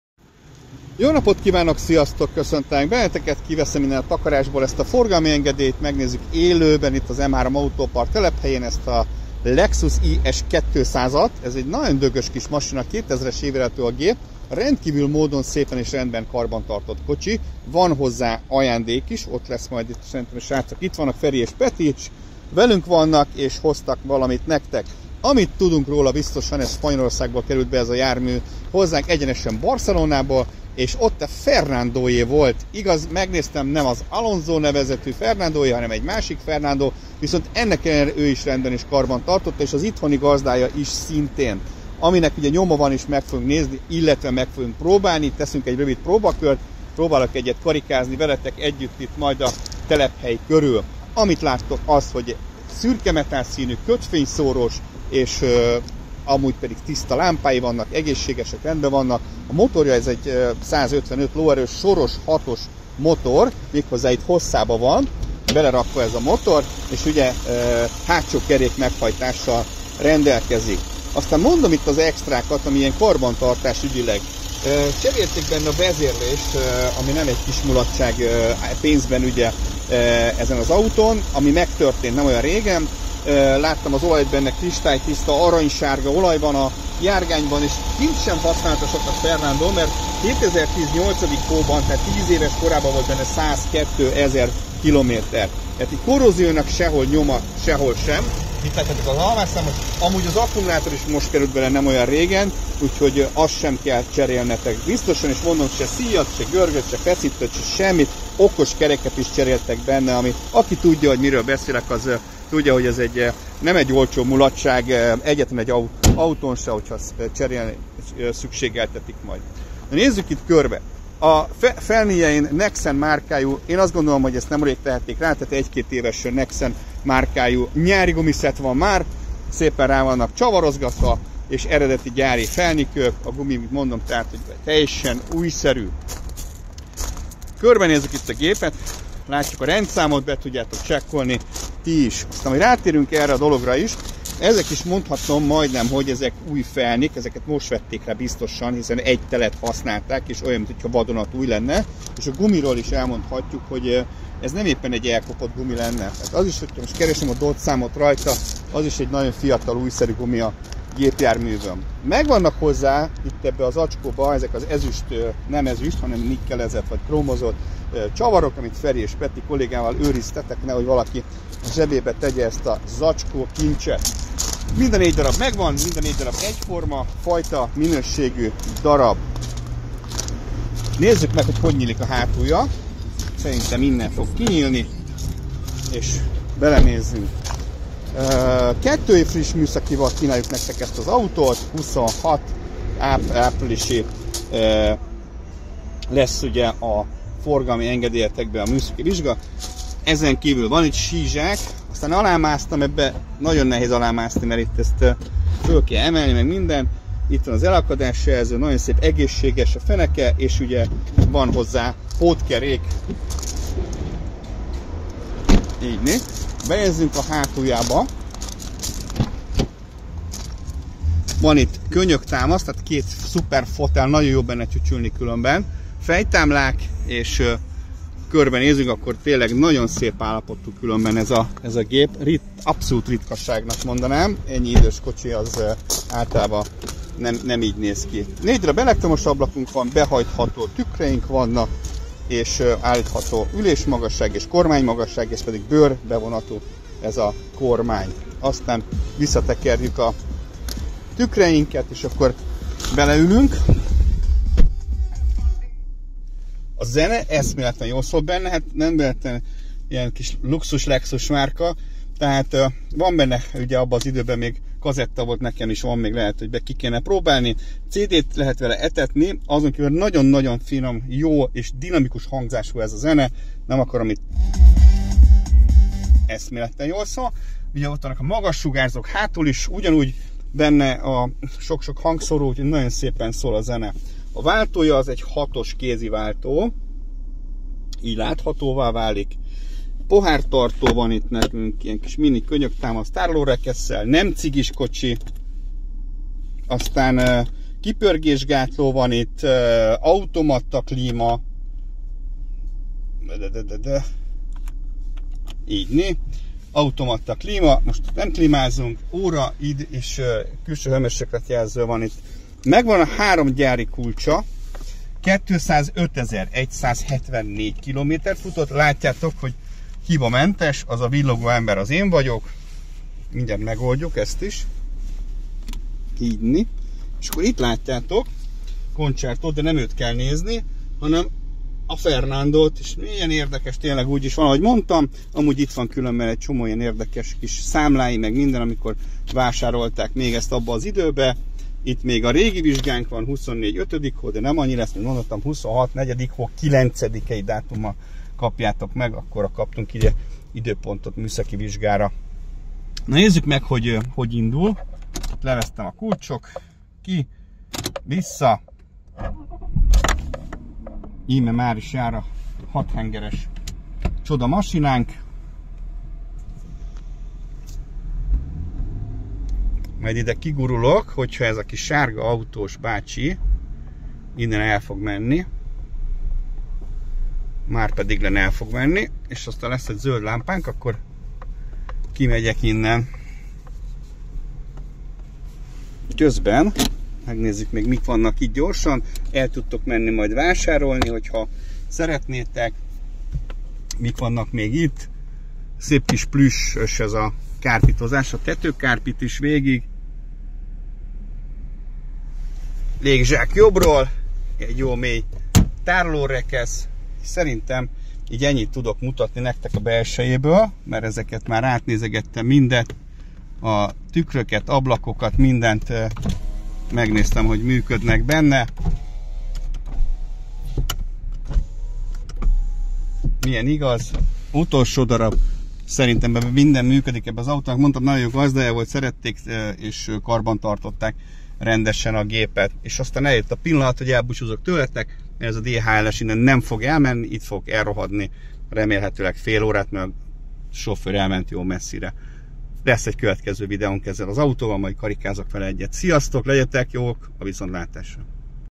Lexus IS200 Teszt - Bemutató - Eladó
Azonnal elvihető!  Garantált Km futású, rendszeresen karbantartott szervizelt Lexus IS 200 eladó erős (155 Le), rugalmas benzines Sor 6-os motorral. Tökéletesen működő, pontos rövid úton közlekedő 6 sebességes kézi váltóval. Hibátlan kopásmentes gyönyörű utastér. Dinamikus hangzású gyári Hifi, CD-Rádió-Magnó. 3 db eredeti gyári kulccsal Jéghideg automata klíma! Újszerű Téli-Nyári gumigarnitúra lemez és gyári alufelniken. 2 év fris műszaki vizsga. Végig vezetett márkaszervizes szervizkönyv! 194 ezer Km-nél komplett vezérlés cserélve! Minden karbantartásról számlák. Minden komfort és biztonsági extra tökéletesen hibátlanúl működik.